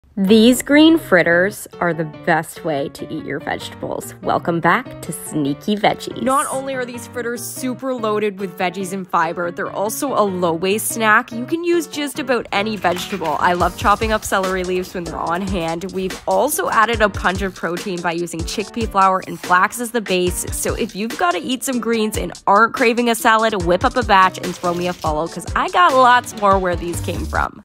eat your greens, kids
These incredible vegan green fritters are going to be your new healthy and high in protein snack recipe. They come together with whatever vegetable you have on hand in your fridge, so they’re naturally low waste as well. Dip them in your favorite sauce (I love vegan tzatziki) and you’ll be in love!